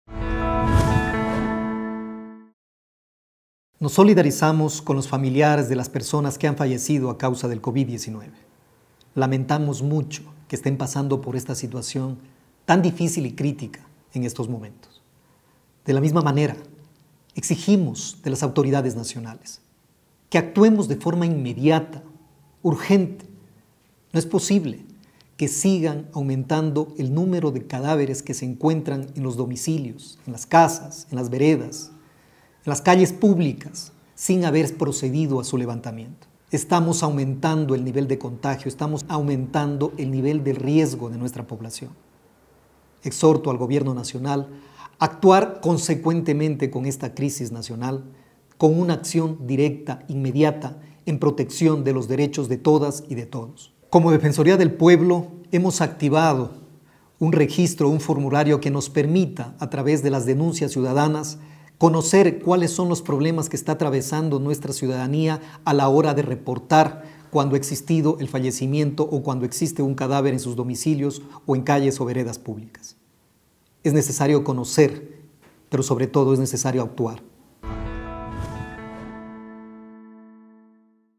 Defensor del Pueblo, Dr. Freddy Carrión Intriago, se pronuncia ante emergencia sanitaria
Defensoría del Pueblo de Ecuador